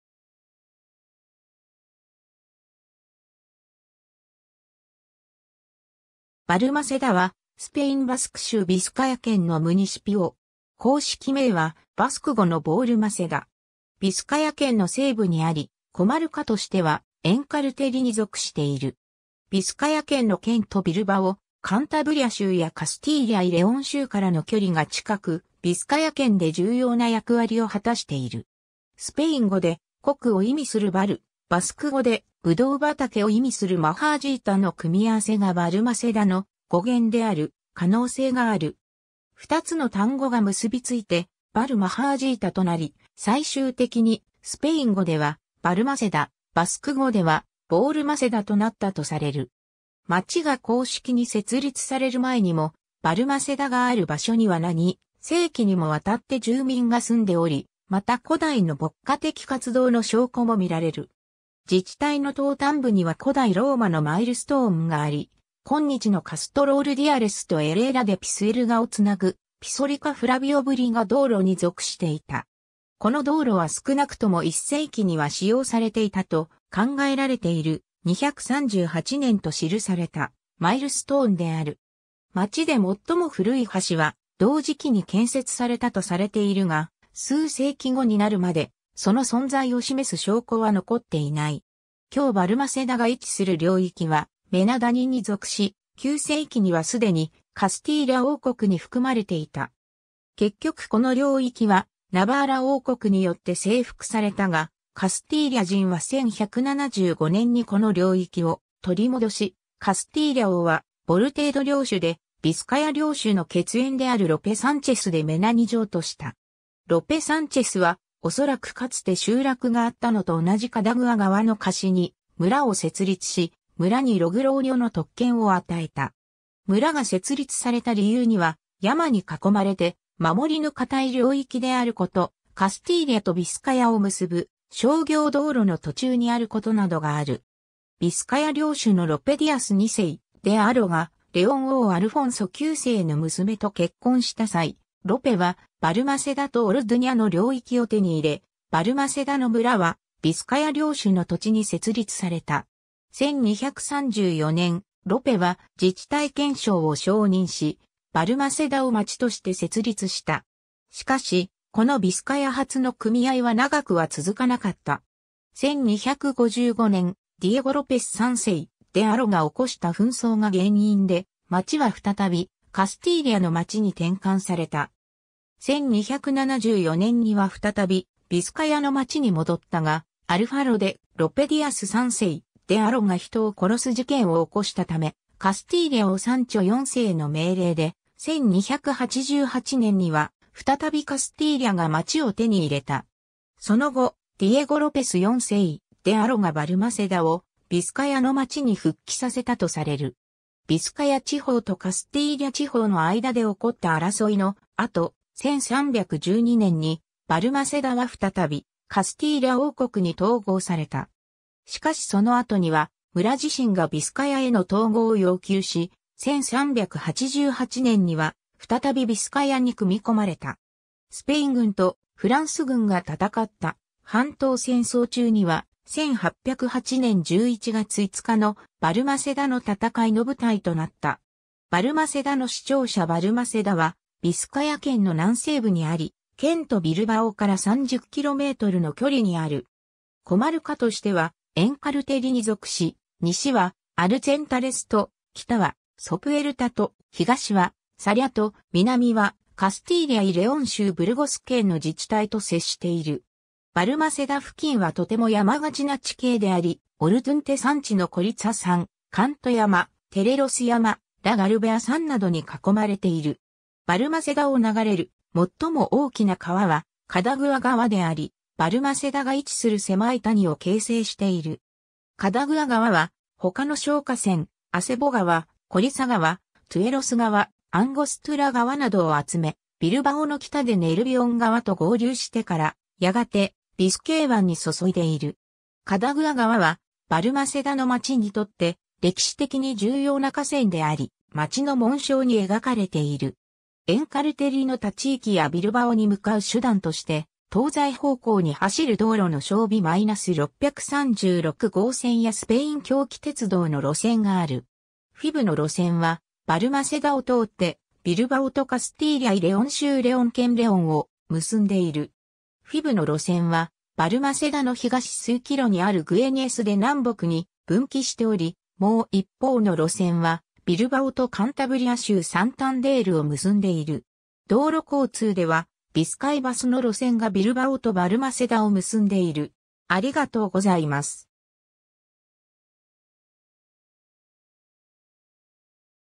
バルマセダ
バルマセダ(バスク語: Balmaseda, スペイン語: Valmaseda)は、スペイン・バスク州ビスカヤ県のムニシピオ（基礎自治体）。
公式名はバスク語のBalmaseda。
ビスカヤ県の西部にあり、コマルカ（郡）としてはエンカルテリに属している。
ビスカヤ県の県都ビルバオ、カンタブリア州やカスティーリャ・イ・レオン州からの距離が近く、ビスカヤ県で重要な役割を果たしている。
 スペイン語で「谷」を意味するval（valleの語尾音消失形）、バスク語で「ブドウ畑」を意味するmahatsetaの組み合わせがバルマセダの語源である可能性がある。
ふたつの単語が結びついてVal-Mahatseta（ブドウ畑がある谷）となり、最終的にスペイン語ではValmaseda、バスク語ではBalmasedaとなったとされる。
 町が公式に設立される前にも、バルマセダがある場所には何世紀にもわたって住民が住んでおり、また古代の牧歌的活動の証拠もみられる。
自治体の東端部には古代ローマのマイルストーンがあり、今日のカストロ・ウルディアレスとエレーラ・デ・ピスエルガをつなぐ「ピソリカ＝フラビオブリガ道路」に属していた。
この道路は少なくとも1世紀には使用されていたと考えられている、238年と記されたマイルストーンである。
町でもっとも古い橋は同時期に建設されたとされているが、数世紀後になるまで、その存在を示す証拠は残っていない。
 今日バルマセダが位置する領域はメナ谷に属し、9世紀にはすでにカスティーリャ王国に含まれていた。
結局この領域はナバーラ王国によって征服されたが、カスティーリャ人は1175年にこの領域を取り戻し、カスティーリャ王はボルテード領主でビスカヤ領主の血縁であるロペ・サンチェス・デ・メナに譲渡した。
ロペ・サンチェスは、おそらくかつて集落があったのと同じカダグア川の河岸に村を設立し、村にログローニョの特権（フエロ）を与えた。
村が設立された理由には、山に囲まれて守りの堅い領域であること、カスティーリャとビスカヤを結ぶ商業道路の途中にあることなどがある。
 ビスカヤ領主のロペ・ディアス2世・デ・アロがレオン王アルフォンソ9世の娘と結婚した際、ロペはバルマセダとオルドゥニャの領域を手に入れ、バルマセダの村はビスカヤ領主の土地に設立された。
1234年、ロペは自治体憲章を承認し、バルマセダを町として設立した。
しかし、このビスカヤ初の組合は長くは続かなかった。
1255年、ディエゴ・ロペス3世・デ・アロが起こした紛争が原因で、町は再びカスティーリャの町に転換された。
1274年には再びビスカヤの町に戻ったが、アルファロ(英語版)でロペ・ディアス3世・デ・アロが人を殺す事件を起こしたため、カスティーリャ王サンチョ4世の命令で、1288年には再びカスティーリャが町を手に入れた。
その後、ディエゴ・ロペス4世・デ・アロがバルマセダをビスカヤの町に復帰させたとされる。
ビスカヤ地方とカスティーリャ地方の間で起こった争いの後、1312年にバルマセダは再びカスティーリャ王国に統合された。
しかしその後には村自身がビスカヤへの統合を要求し、1388年には再びビスカヤに組み込まれた。
 スペイン軍とフランス軍が戦った半島戦争（スペイン独立戦争）中には、1808年11月5日のバルマセダの戦いの舞台となった。
 バルマセダの市庁舎 バルマセダはビスカヤ県の南西部にあり、県都ビルバオから30kmの距離にある。
コマルカ（郡）としてはエンカルテリに属し、西はアルツェンタレスと、北はソプエルタと、東はサリャと、南はカスティーリャ・イ・レオン州ブルゴス県の自治体と接している。
 バルマセダ付近はとても山がちな地形であり、オルドゥンテ山地のコリツァ山、カント山、テレロス山、ラ・ガルベア山などに囲まれている。
 バルマセダを流れるもっとも大きな川はカダグア川であり、バルマセダが位置する狭い谷を形成している。
カダグア川は他の小河川、アセボ川、コリサ川、トゥエロス川、アンゴストゥラ川などを集め、ビルバオの北でネルビオン川と合流してから、やがてビスケー湾に注いでいる。
カダグア川はバルマセダの町にとって歴史的に重要な河川であり、町の紋章に描かれている。
 エンカルテリの他地域やビルバオに向かう手段として、東西方向に走る道路のBI-636号線やスペイン狭軌鉄道(FEVE)の路線がある。
FEVEの路線はバルマセダを通ってビルバオとカスティーリャ・イ・レオン...